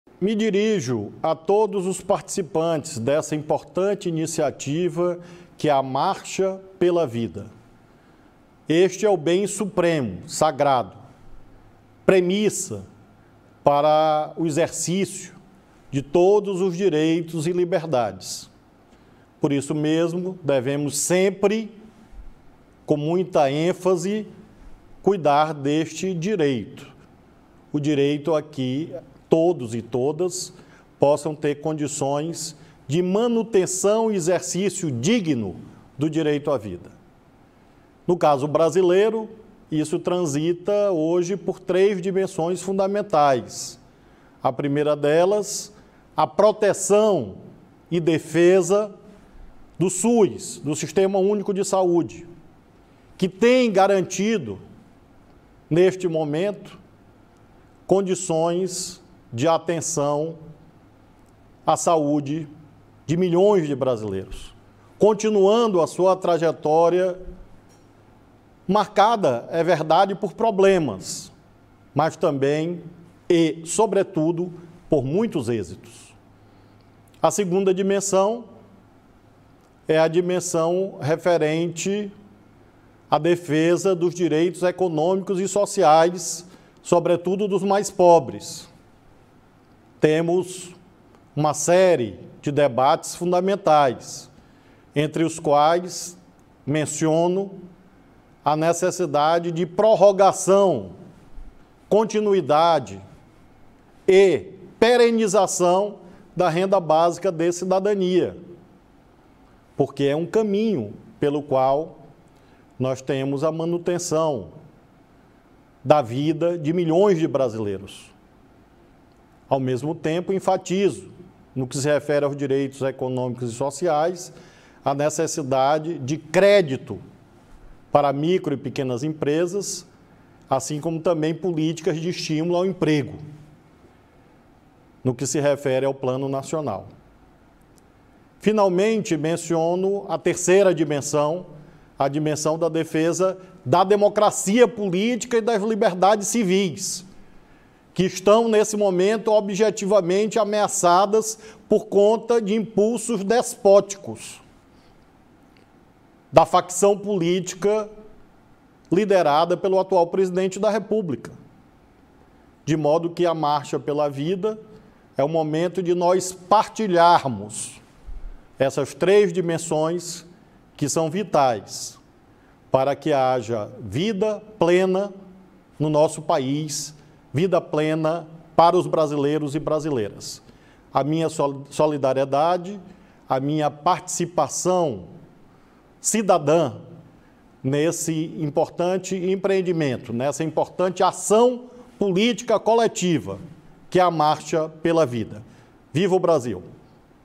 Depoimento de Flávio Dino em favor da
Por meio de um tuitaço com as hashtags MarchaPelaVida e FrentePelaVida, manifestantes em todo o país expressaram a importância do movimento que defende a vida, com a participação de diferentes setores da sociedade brasileira.